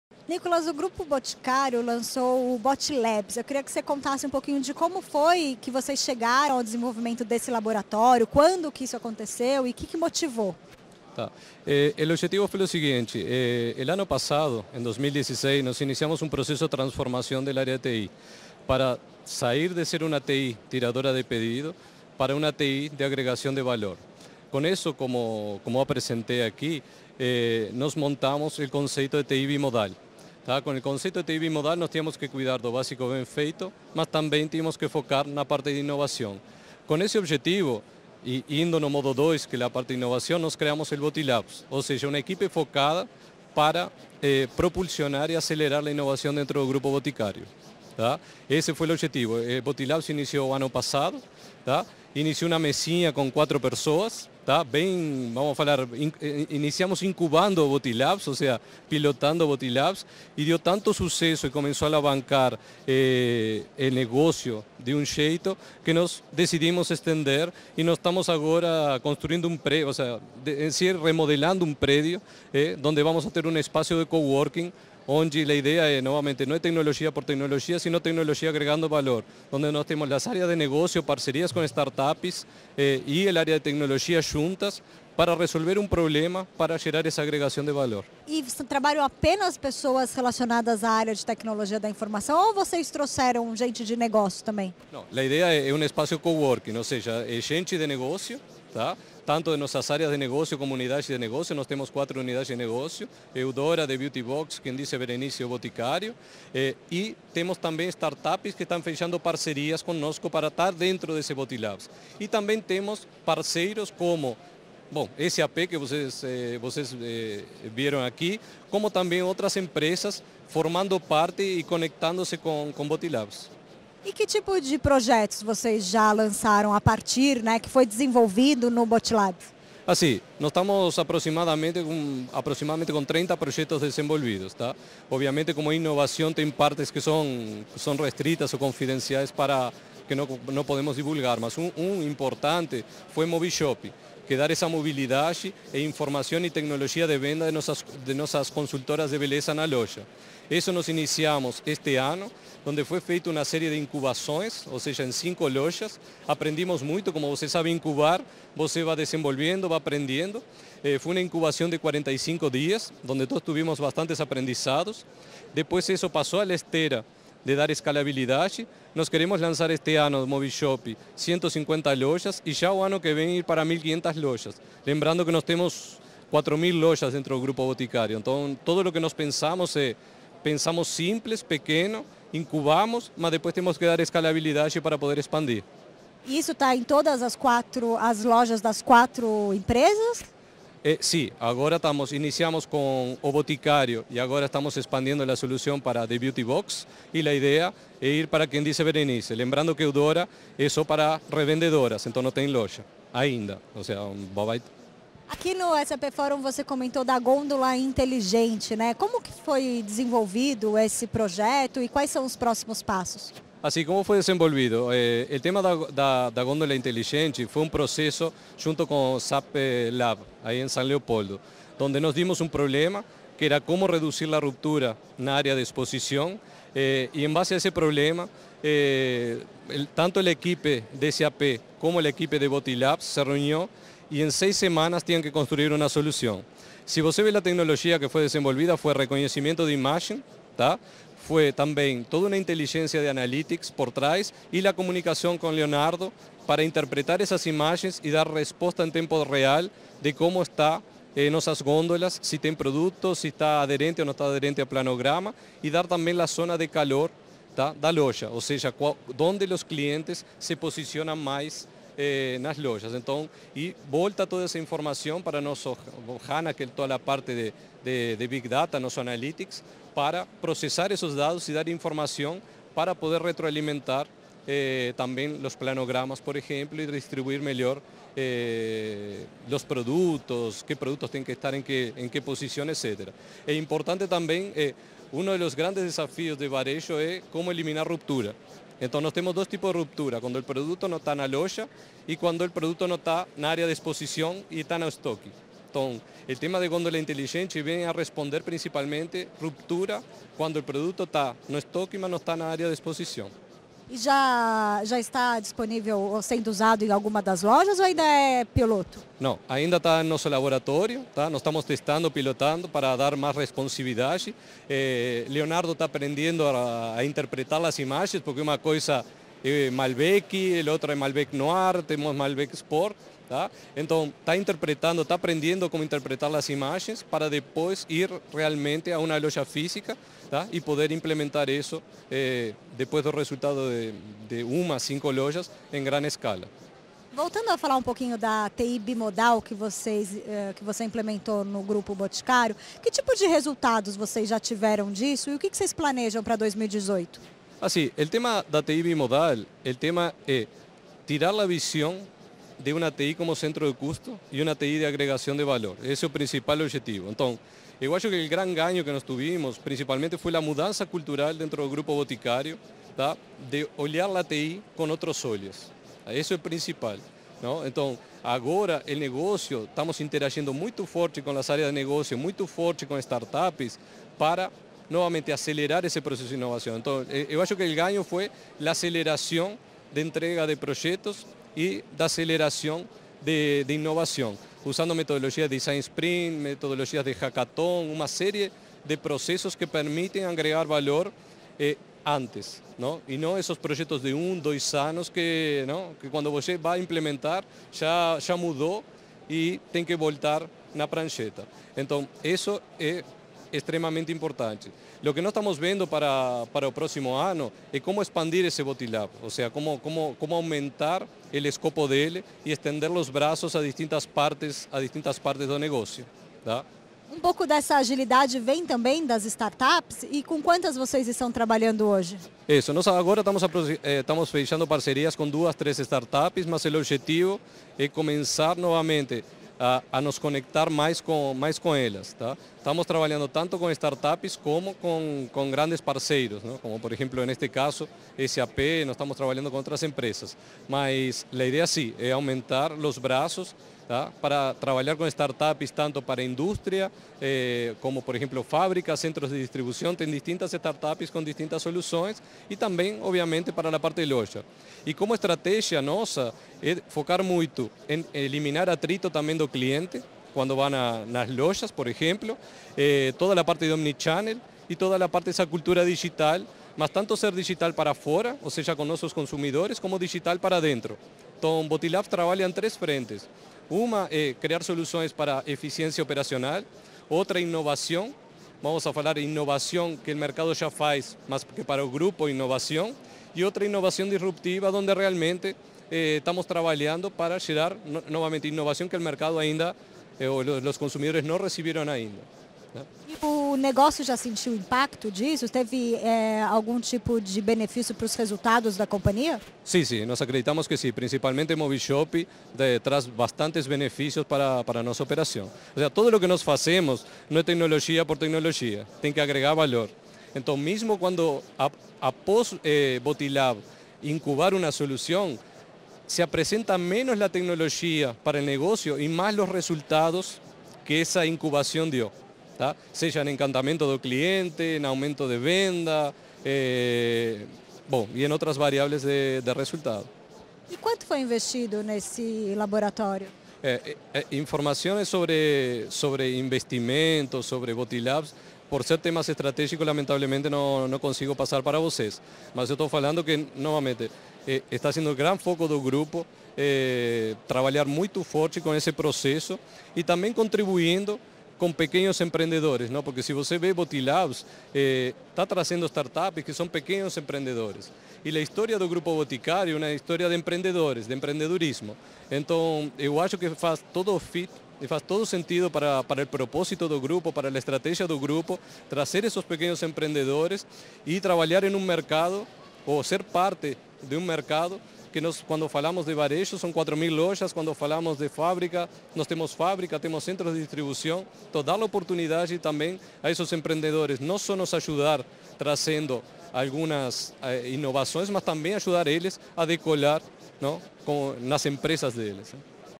Grupo Boticário reforça uso de dados e cria gôndola inteligente
O desafio do Grupo Boticário de desenvolver e apoiar a nova cultura digital levou a holding, formada por quatro empresas — O Boticário; Eudora; Quem disse, Berenice?; e The Beauty Box...